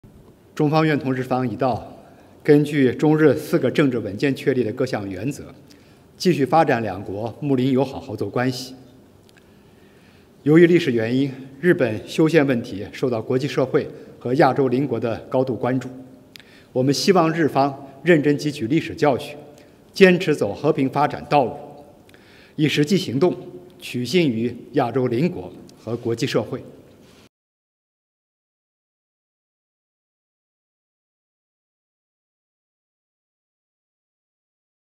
China hopes Japan draw lessons from history, stick to peaceful development in amending Constitution
China on Monday called on Japan to draw serious lessons from history and adhere to the path of peaceful development.

Japan's ruling bloc of the Liberal Democratic Party (LDP) and Komeito scored a sweeping victory in Sunday's House of Councilors election, with the LDP alone securing a majority of contested seats, according to the election results published on Monday morning. The election results mean that the pro-Constitutional amendment forces take up over two thirds of the seats in Japan's upper house.

Wang Wenbin, a spokesman of China's Ministry of Foreign Affairs, called on Japan to adhere to the path of peaceful development at a press conference in Beijing.

"China is ready to work with Japan to continue developing good-neighborly, friendly, and cooperative bilateral relations by following the principles set out in the four political documents between the two countries. Because of historical reasons, the amendment of Constitution in Japan is followed closely by the international community, especially Japan's Asian neighbors. We urge Japanese side to draw serious lessons from history, adhere to the path of peaceful development, and win the trust from its Asian neighbors and the international community with concrete actions," said Wang.